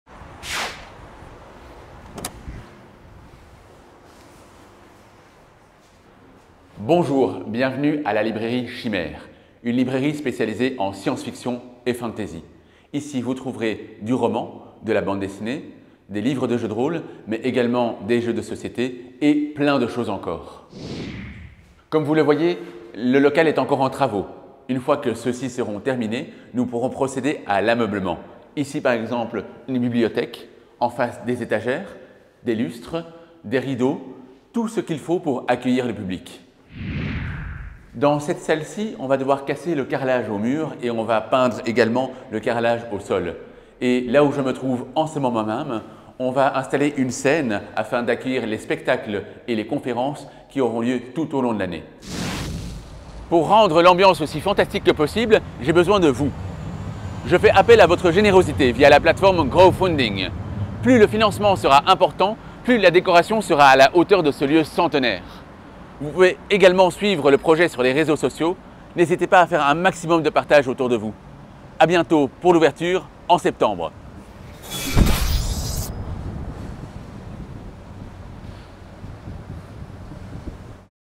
Librairie Chimères
La librairie Chimères est un espace de rencontres pour tou-te-s les passionné-e-s des mondes de l’imaginaire. Ici, vous pourrez profiter des conseils avisés du libraire, des œuvres les plus récentes aux pépites oubliées. Venez également découvrir l’ambiance unique du lieu lors de soirées thématiques !